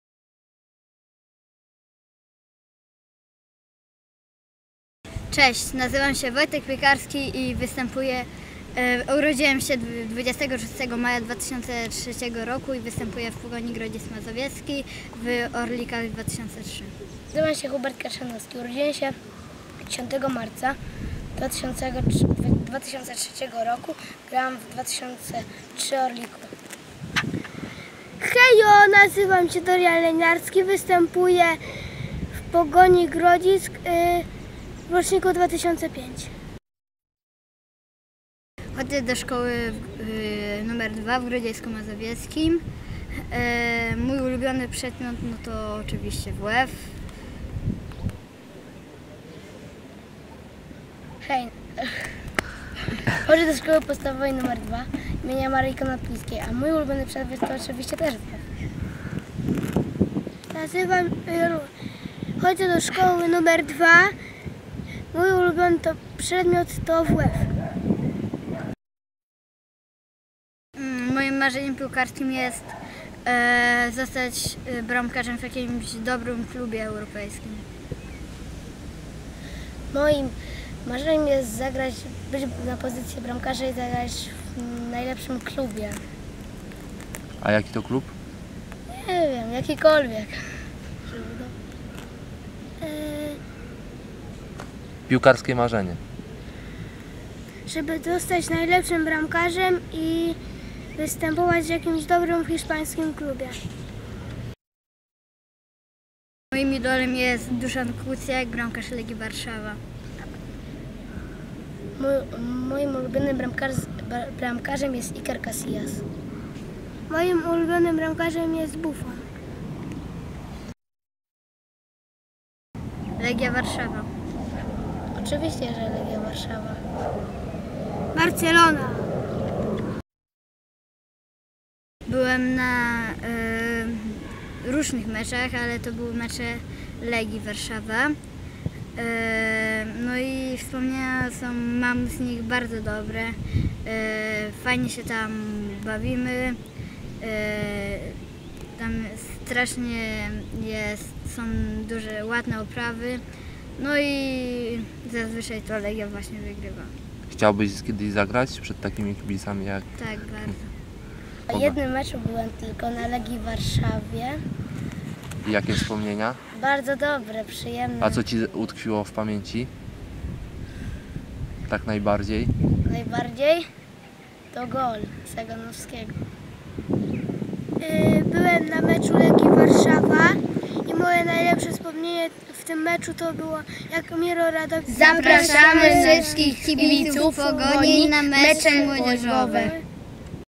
Pogoń TV: Poznajcie nasze pociechy.
Pogoń Grodzisk to nie tylko pierwszy zespół seniorów, ale również nasze dzieciaczki, które wylewają pot a czasami nawet łzy za swój ukochany klub :)